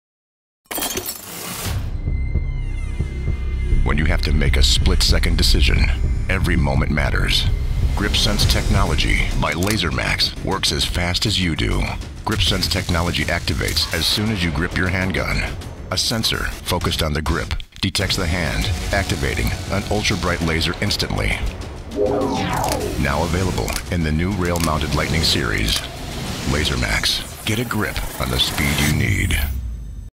LaserMax Lightning Series with GripSense
Our video partner Jeff Voves created this 30 second video for LaserMax to showcase how their GripSense Technology senses a hand on the grip and automatically activates an ultra bright laser in the new rail mounted Lighting Series.